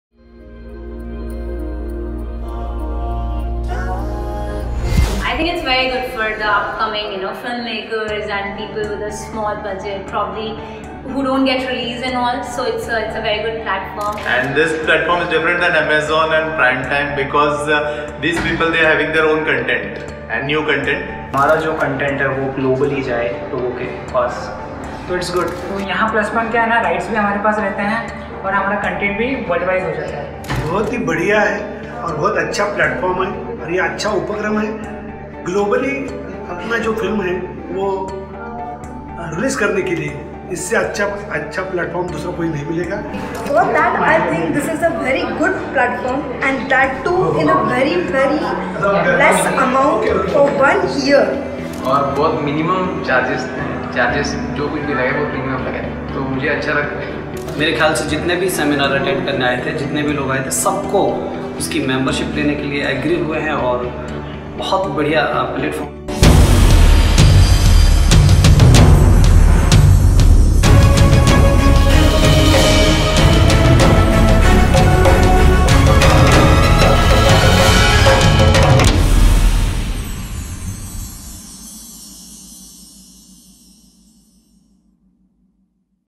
ATB SHOWTIME OTT Platform Mumbai Launch | OTT Seminar Mumbai Reviews
Tame the chaos of releasing a movie with a simple platform. What well-known producers and directors from Industry say about us.

After a successful event at Mumbai, We are teaming up for Kolkata.